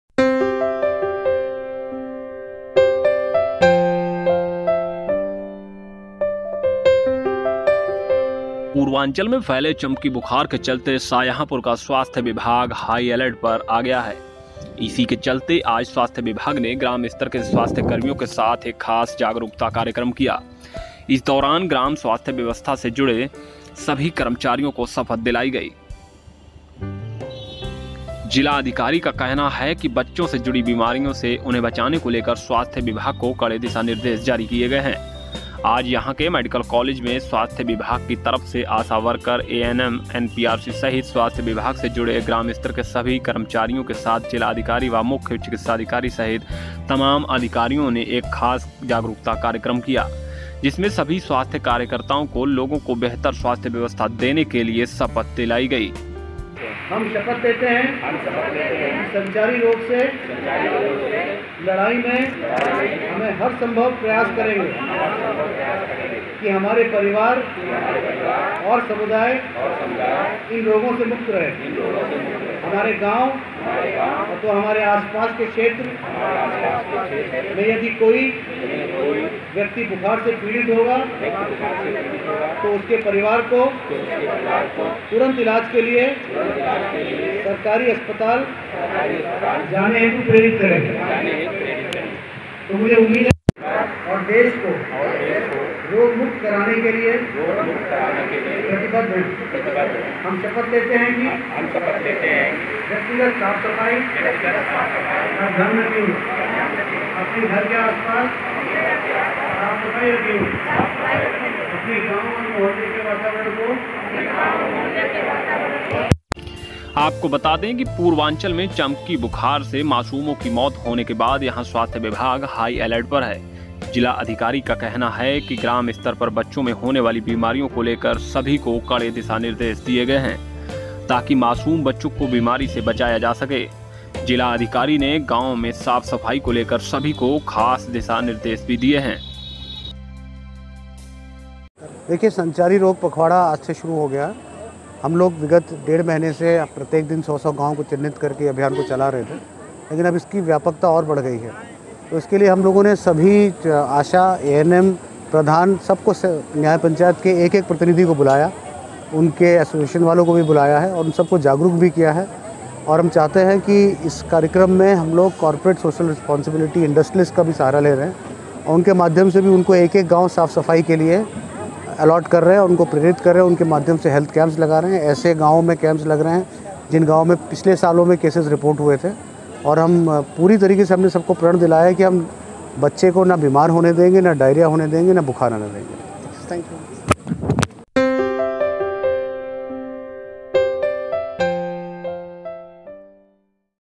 संचारी रोगों पर अब होगा अंकुश
शाहजहांपुर में संचारी रोगों की रोकथाम के लिए जिलाधिकारी ने स्वास्थ्य कर्मियों को शपथ दिलाई गई।